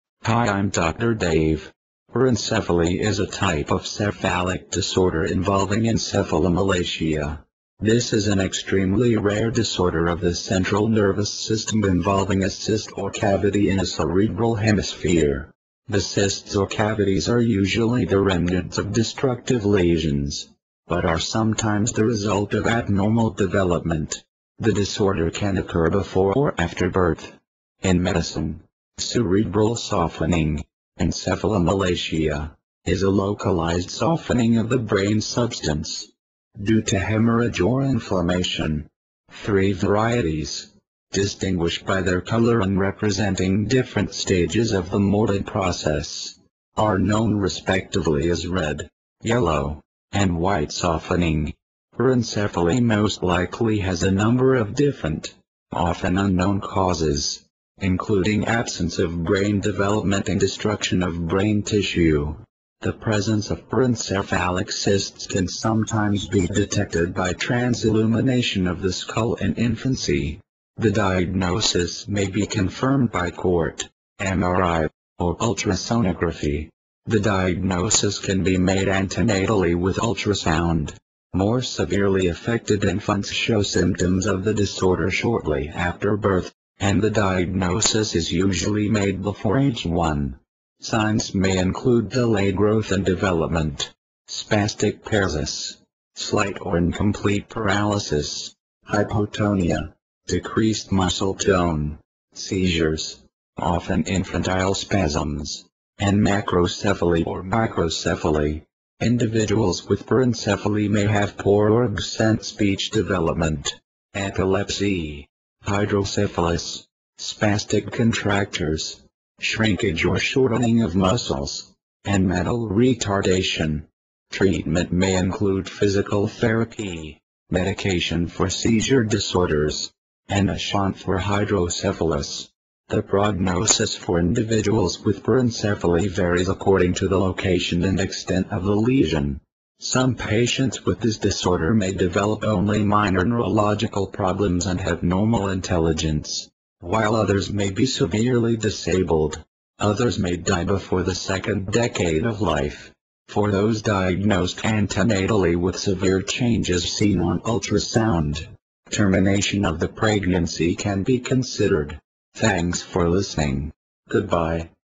Porencephaly
Learn a little bit about Porencephaly. Learn about its treatment, diagnosis, and what exactly is it.